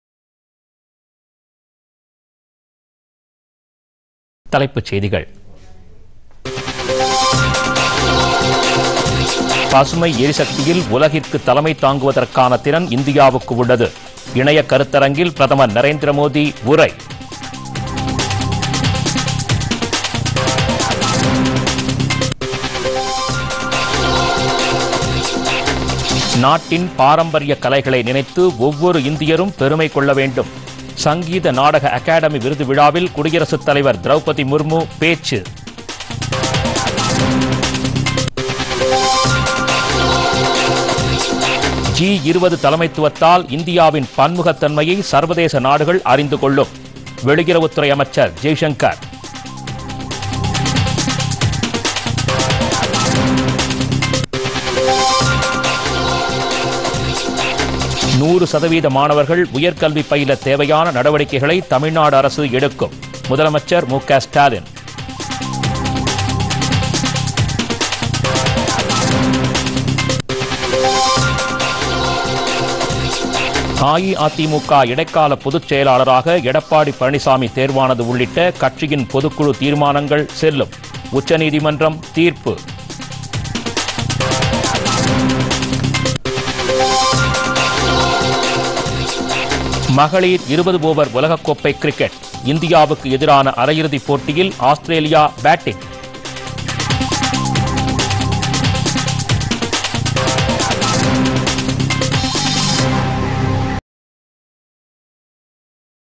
பொதிகை இரவு 7.00 மணி தலைப்புச்செய்திகள் [23.02.2023]PodhigaiTamilNews
1)  பசுமை எரிசக்தியில் உலகிற்கு தலைமை தாங்குவதற்கான திறன் இந்தியாவுக்கு உள்ளது - இணைய கருத்தரங்கில் பிரதமர் நரேந்திர மோதி உரை.

2)   நாட்டின்  பாரம்பரியக் கலைகளை நினைத்து ஒவ்வொரு இந்தியரும் பெருமை கொள்ள வேண்டும்  - சங்கீத நாடக அகாடமி விருது விழாவில் குடியரசுத் தலைவர் திரவுபதி முர்மு பேச்சு.

3)  ஜி20 தலைமைத்துவத்தால் இந்தியாவின் பன்முகத்தன்மையை சர்வதேச நாடுகள் அறிந்து கொள்ளும்  - வெளியுறவுத்துறை அமைச்சர் ஜெய்சங்கர்.

4)  100 சதவீத மாணவர்கள்  உயர்கல்வி பயில தேவையான நடவடிக்கைகளை தமிழ்நாடு அரசு எடுக்கும் - முதலமைச்சர் மு க ஸ்டாலின்.

5)   அஇஅதிமுக இடைக்கால பொதுச்செயலாளராக எடப்பாடி பழனிசாமி தேர்வானது உள்ளிட்ட  கட்சியின் பொதுக்குழு தீர்மானங்கள் செல்லும் - உச்சநீதிமன்றம் தீர்ப்பு

6)    மகளிர் 20 ஓவர் உலகக் கோப்பை கிரிக்கெட்  - இந்தியாவுக்கு எதிரான அரையிறுதிப் போட்டியில் ஆஸ்திரேலியா  பேட்டிங்.